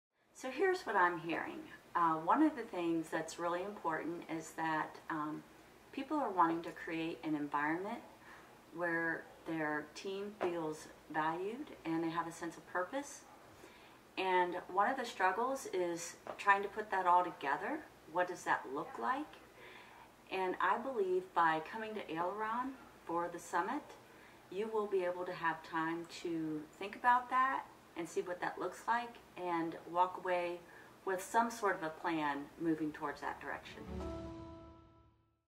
A Piece from Lois Elrich: Aileron Summit
Feeling stuck? Hear from Lois Elrich, Business Advisor, on what AileronSummit can do for you and your business to help you work through that standstill.